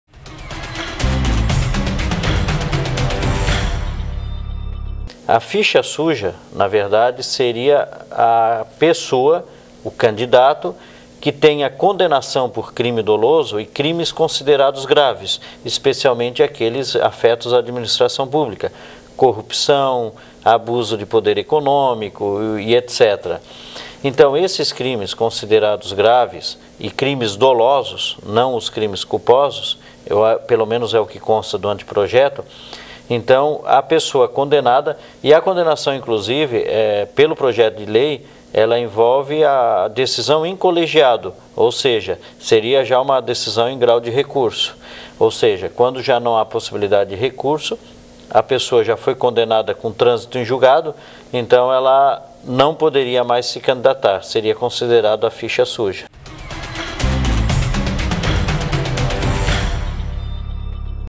Direito na TV: ficha suja de candidatos
As regras antes e durante as eleições. No quadro O direito na TV de hoje, o Juiz eleitoral da comarca de Tubarão, Eron Pizzolati, fala sobre ficha suja dos candidatos